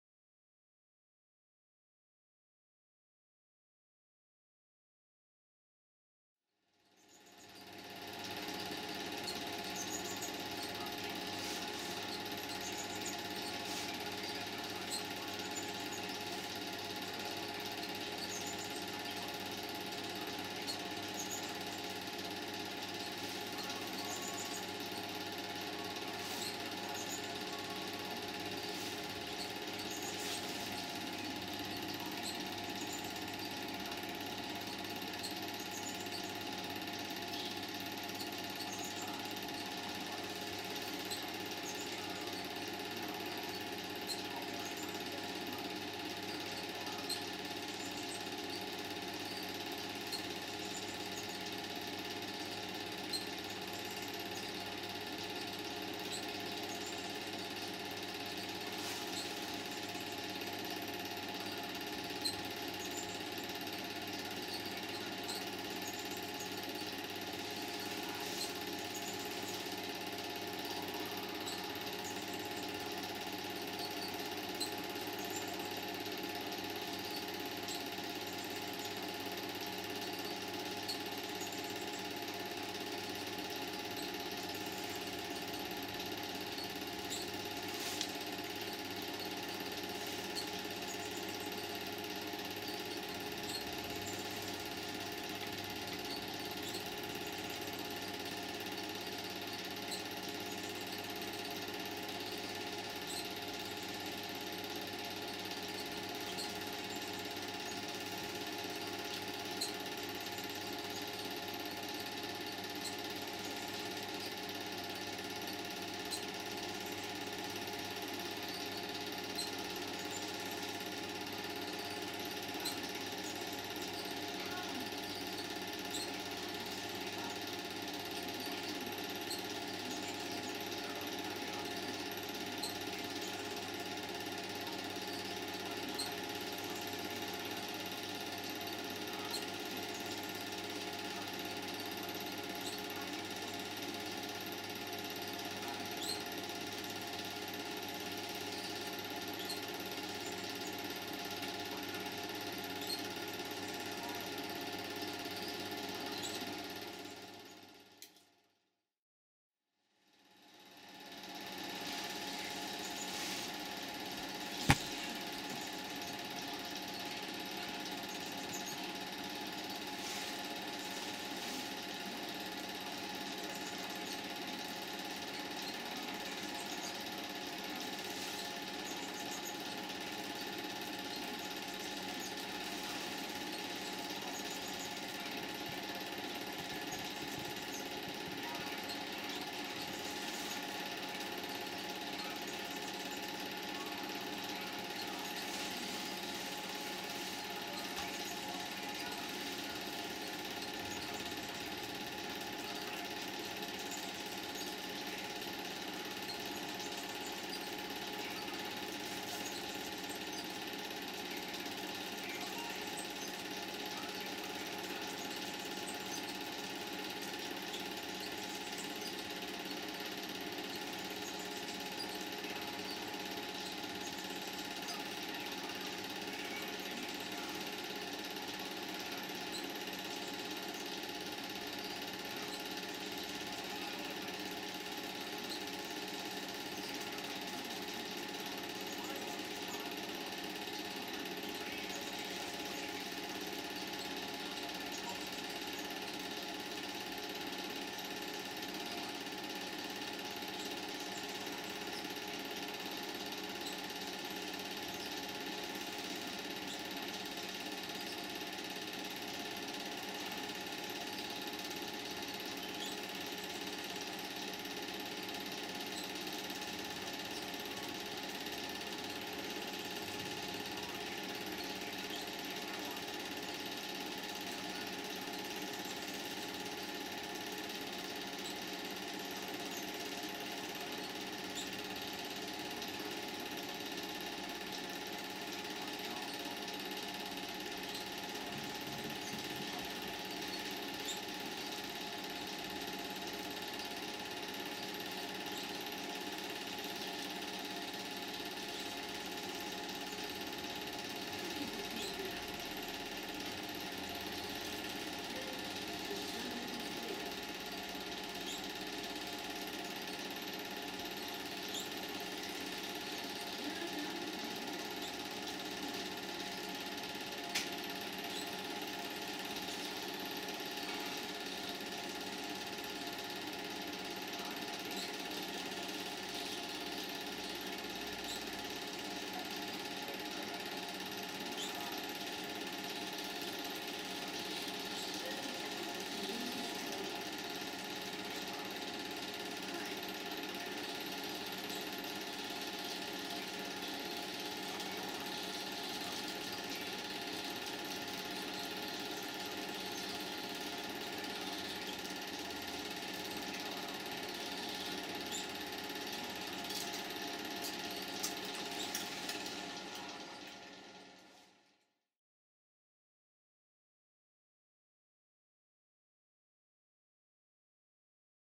Two of my Super 8 Home Movies (from circa 1970?)
PLEASE NOTE THESE ARE SILENT FILMS (apart from some projector noise)

After digging out my old super 8 projector (model Eumig Mark S) I was surprised by how well it still worked. Unfortunately I only managed to record these two of my old super 8 films before the projector lamp died (grrr!) also the spare I had blew the instant I tried it (sigh).
The first film features scenes of (I think?) the Matlock Show, Cromford canal in Derbyshire and some animals on the Hardwick Hall estate.
The second features my mother, my sister and two of my sister's grand children.
Comments welcomed - especially frm anyone recognising themselves on the footage. ^^